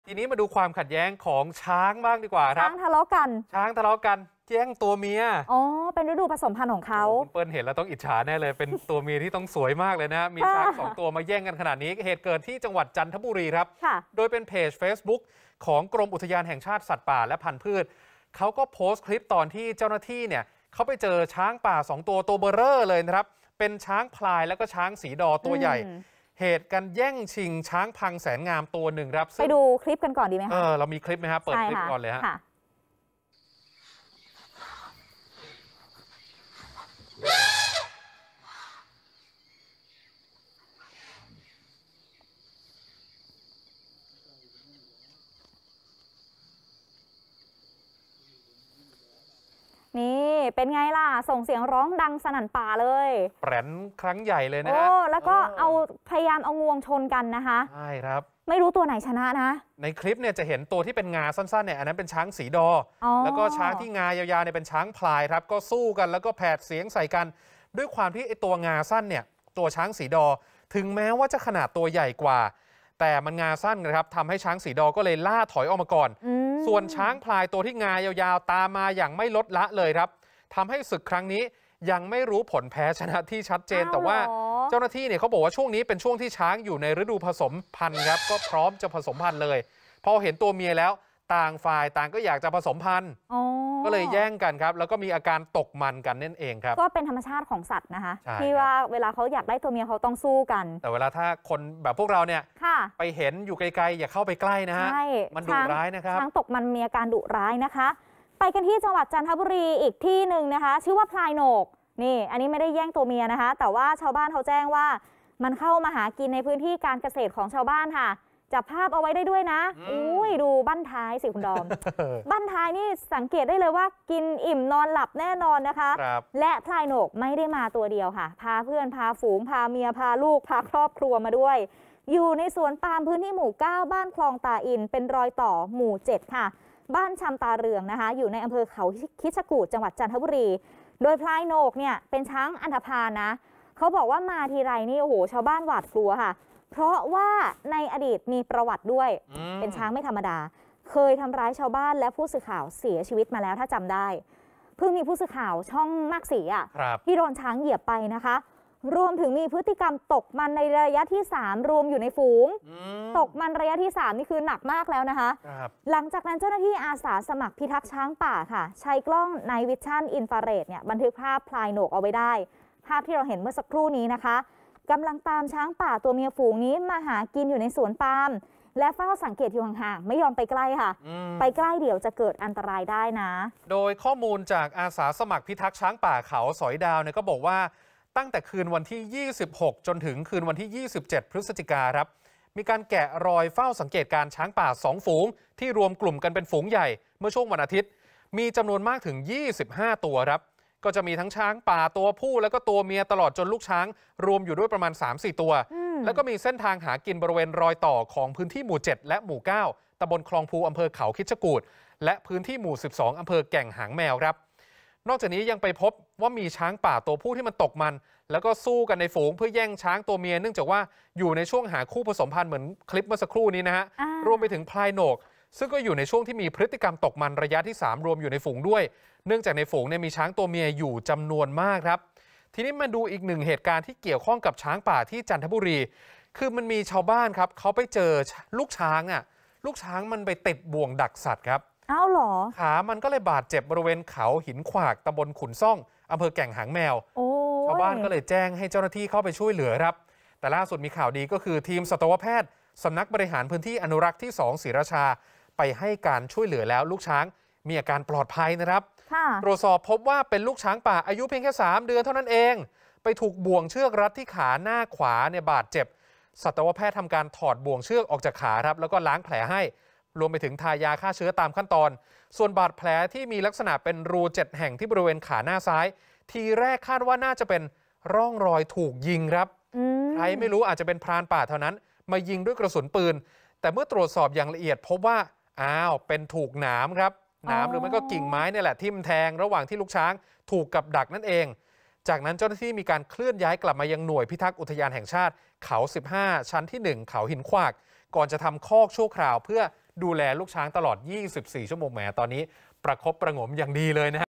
เปิดคลิปช้างป่าเขาสอยดาวเปิดศึกแย่งความเป็นใหญ่
เฟซบุ๊กกรมอุทยานแห่งชาติ สัตว์ป่า และพันธุ์พืช เผยแพร่คลิปนาทีศึกช้างชนช้าง ระหว่างช้างพลายและช้างสีดอตัวใหญ่ต่อสู้กัน บริเวณหลังหน่วยพิทักษ์ป่าวังแพร จ.จันทบุรี สาเหตุอาจมาจากการแย่งความเป็นใหญ่ เพราะช่วงนี้ช้างอยู่ในฤดูผสมพันธุ์ มีอาการตกมัน และแย่งชิงช้างพังตัวเมีย